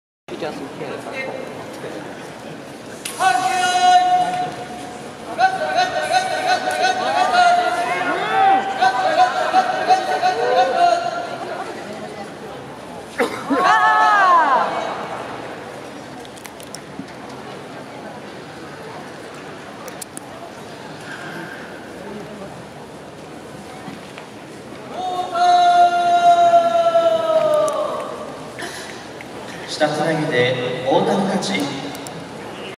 Matsuda (4-3) - Ota (5-2) , Sumo : Natsubasho '17
(l) Matsuda - Jd 21e, Minezakibeya
(r) Ota - Jd 24w, Yamahibikibeya
Kimarite : Shitatenage